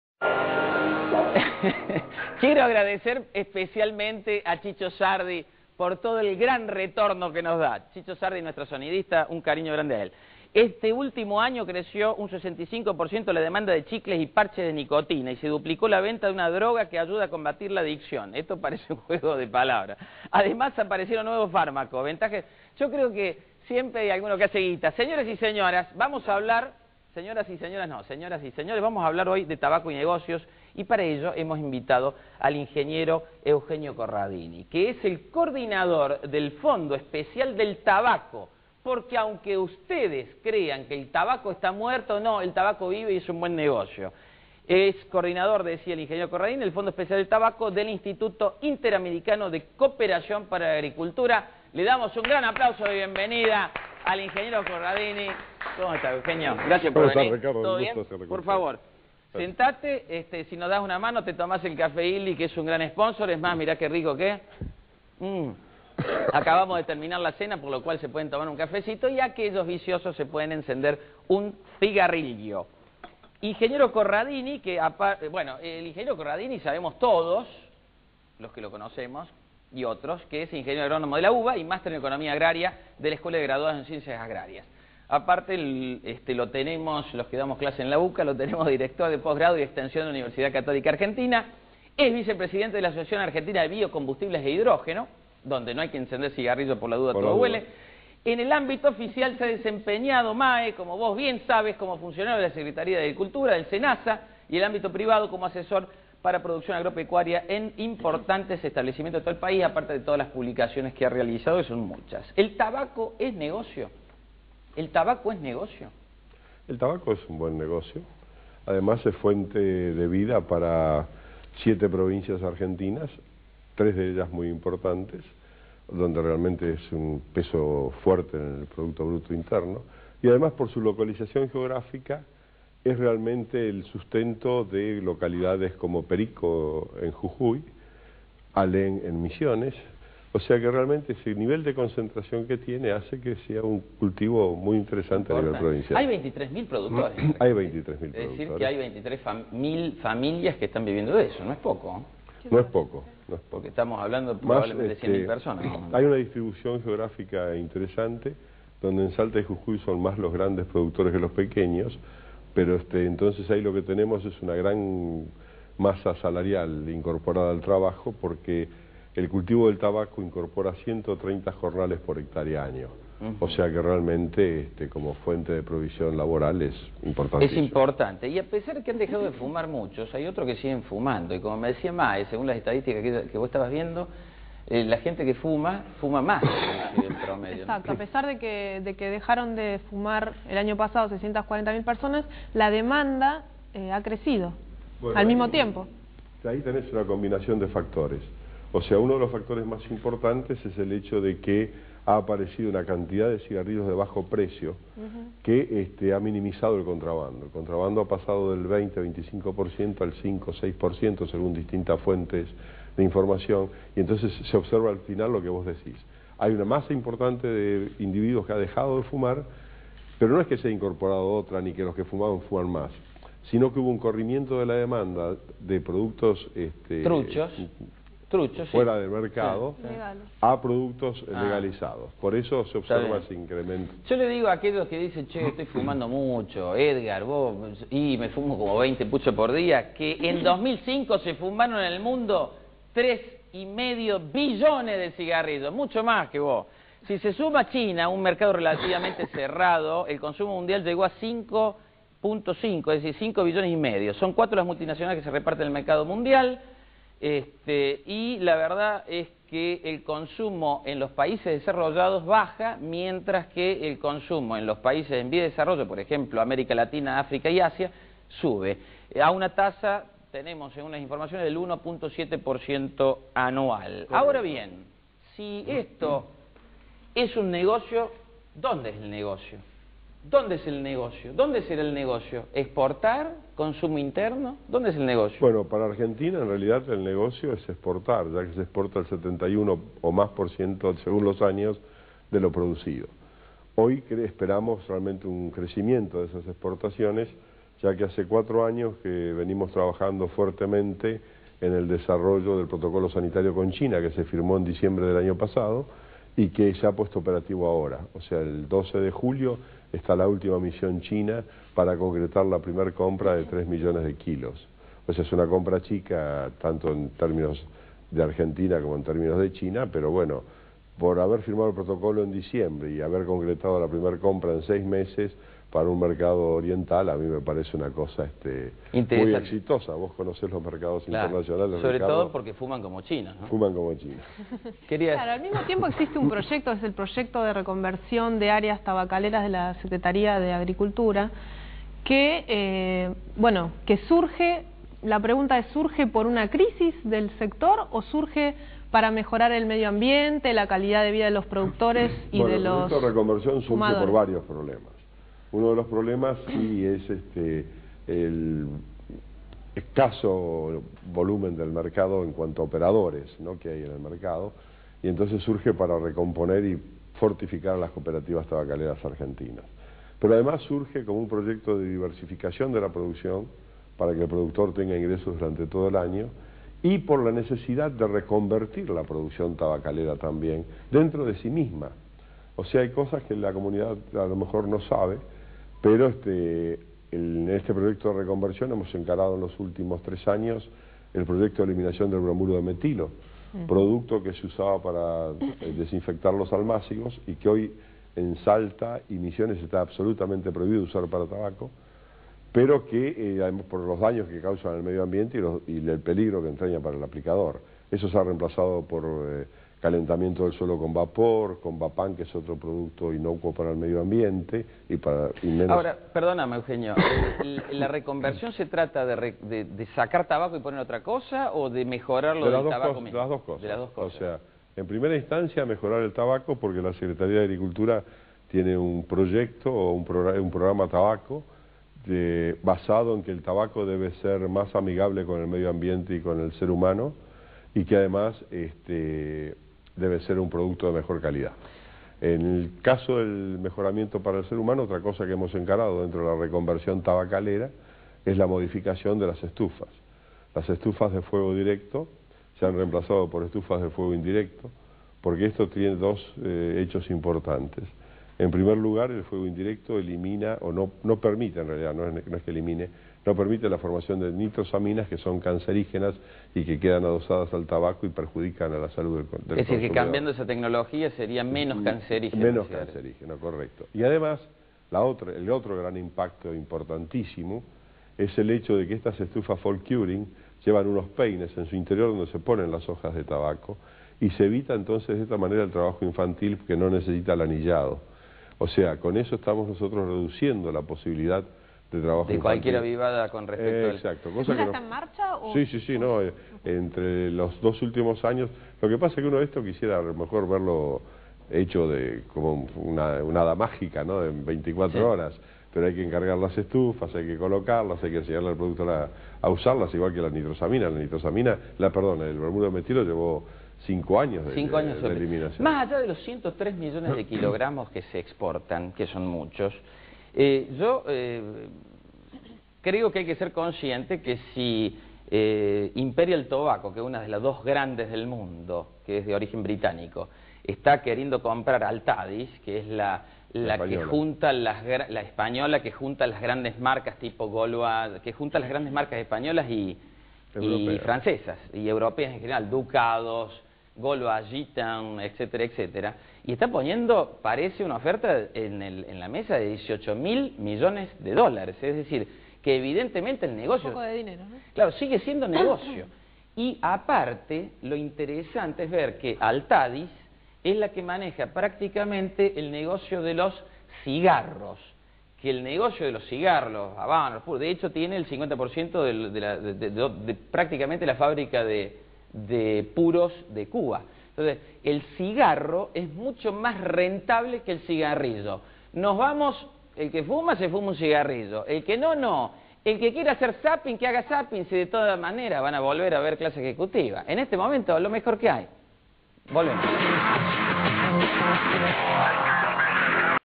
Clase Ejecutiva "Tabaco y Negocios" 1º Parte
Invitado: Ing. Eugenio Corradini. Coord. General Inst. Interamericano de Cooperación para la Agricultura - Fondo Especial del Tabaco.